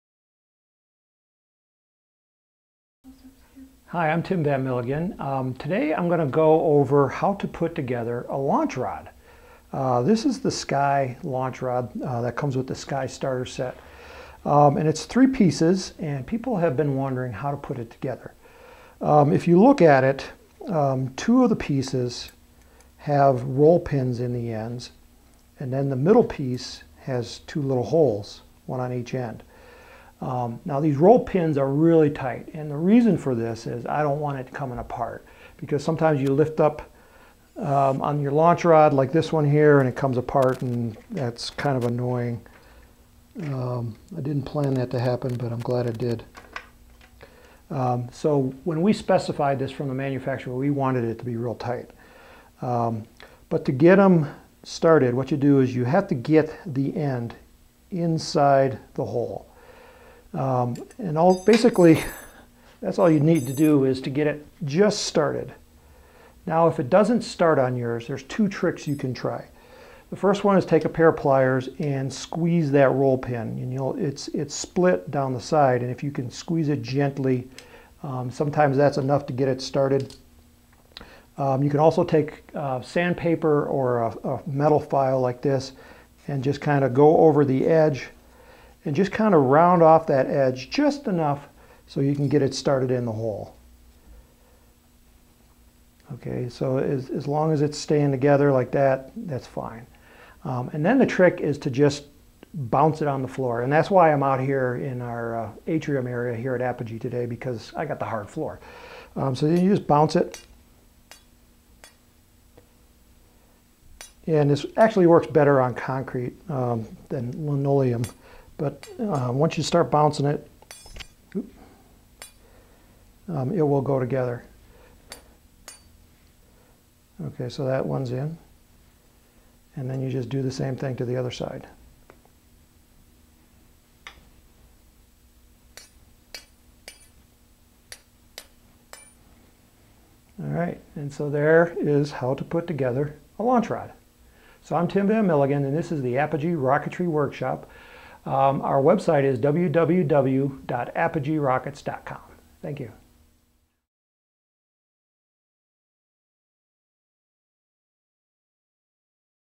How To Assemble A Launch Rod
| Many people that are just starting in rocketry are wondering how to put together the 3-piece launch rod that comes with our starter sets. The reason they ask is that the pins on the ends of the rods are a bit tight. I like them like that, because then the rod doesn't fall apart when you're moving the launch pad around on the field. So in this week's video, I'm going to show you a couple of tricks that you can use to get the rod together in just a few seconds.